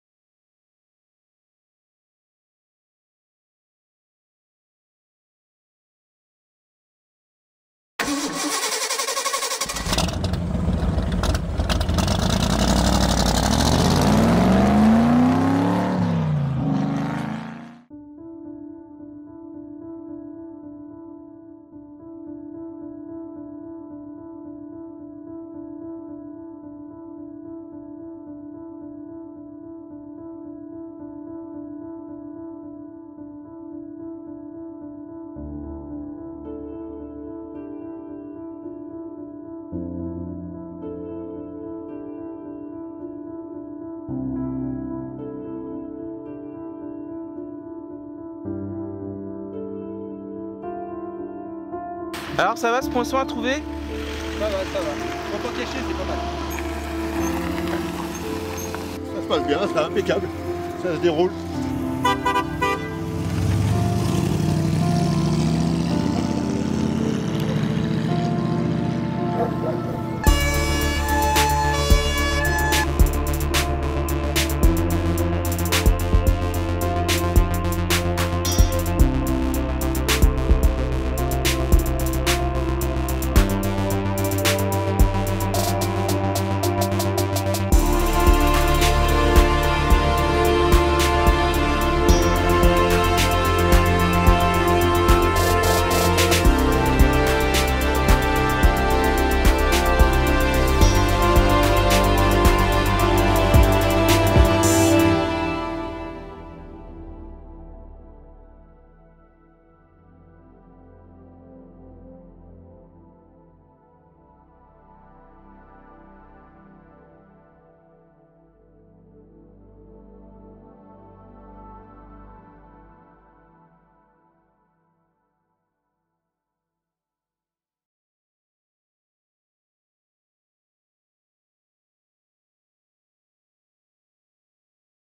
[OFFICIAL] Clémentitine 8 Teaser 2022 - Saint Clément Sur Valsonne
Nous sommes de retour en 2022 pour une nouvelle Clémentitine à Saint Clément sur Valsonne !

Vidéo : Prise des Clémentitines précédente et de vidéo libre de droit